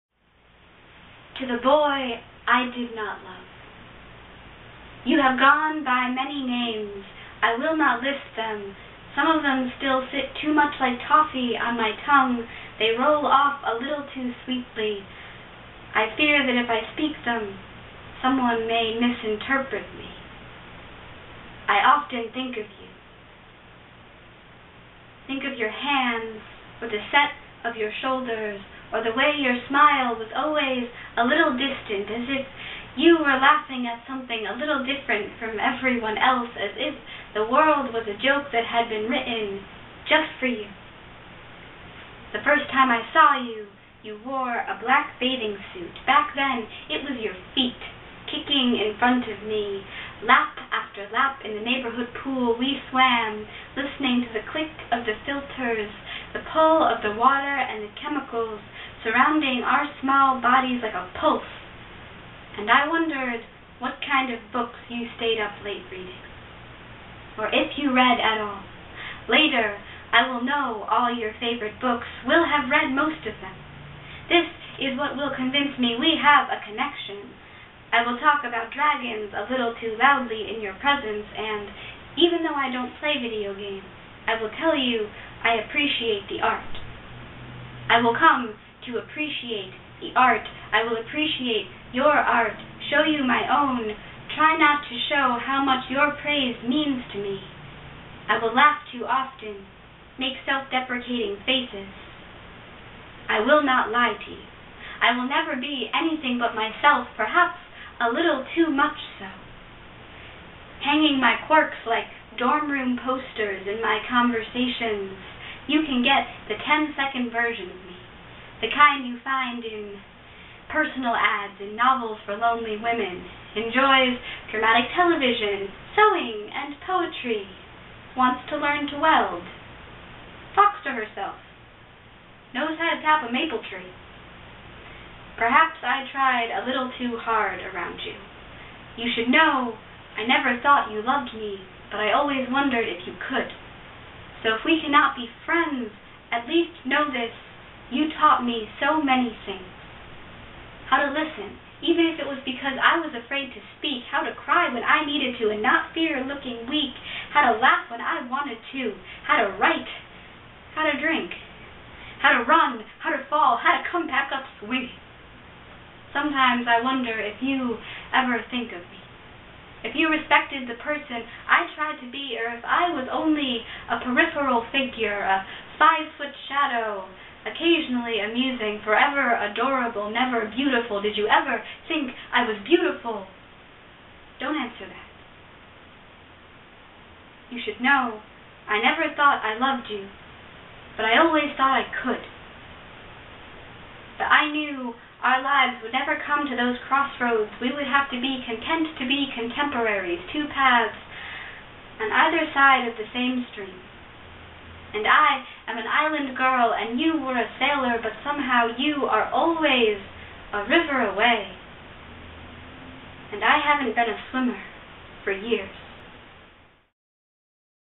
To the Boy I Did Not Love (original poem)
So it seems I'm into quasi love poetry as of late. This is a poem not really to or about one boy, but many. It's a poem to the idea of a crush, and how its influence has carried through my experiences.